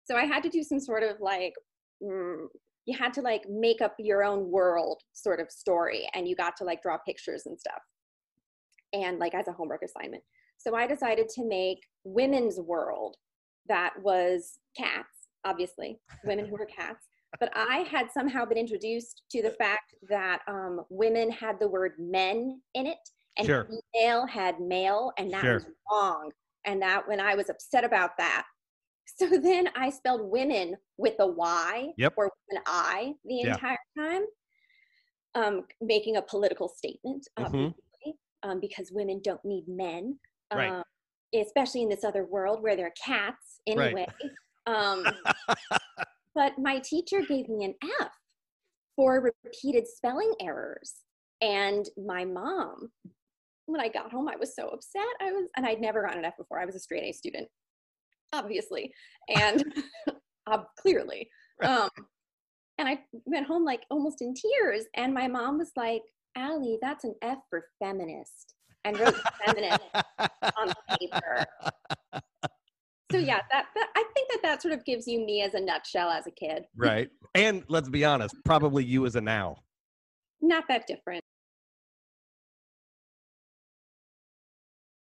Episode 76: Alisson Wood—Womyn's World
On Monday, Aug. 3, we've got Alisson Wood on the program talking about her memoir, BEING LOLITA.

This was a hard, harrowing interview. But those moments don't define Alisson. Here's my favorite part of the show! And, a pretty good indication of why she ended where she is today.